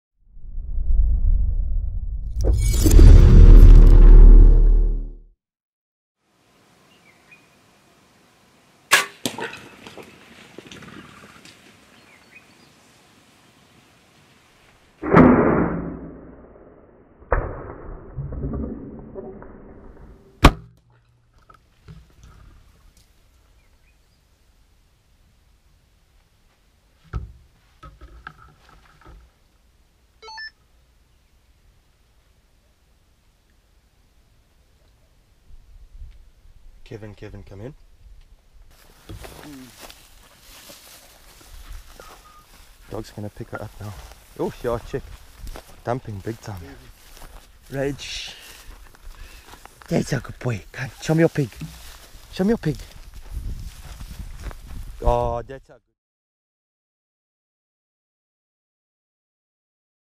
SAHJ - Late Season Pot Pig For The Land Owner
Warthog, Gravy pig for the freezer I hunted this pig in Late August. (Find the full hunt on Episode 6)